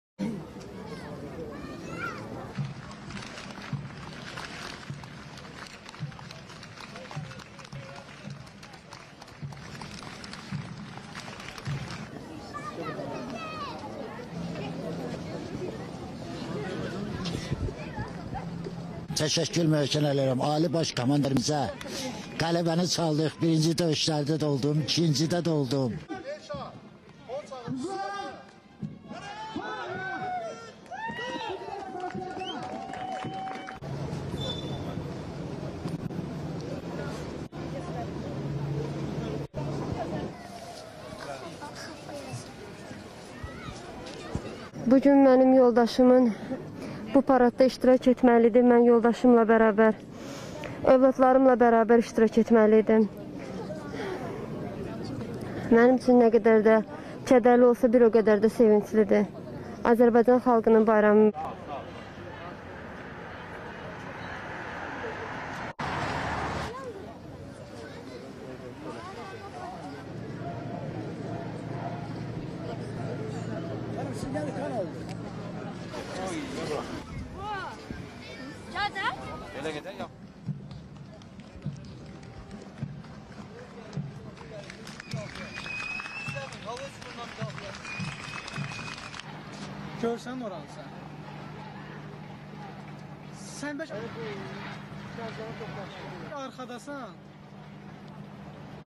Azerbaycan'ın Karabağ zaferinin 1. yılında
Azebaycan'ın Karabağ Zaferi'nin birinci yıldönümü Pazartesi günü Bakü'de kutlandı. Askeri bandonun marşları ile başlayan yürüyüşte askeri lise öğrencileri, savaşın 44 gün sürmesine atıfta bulunulan 440 metrelik Azerbaycan bayrağını taşıdı.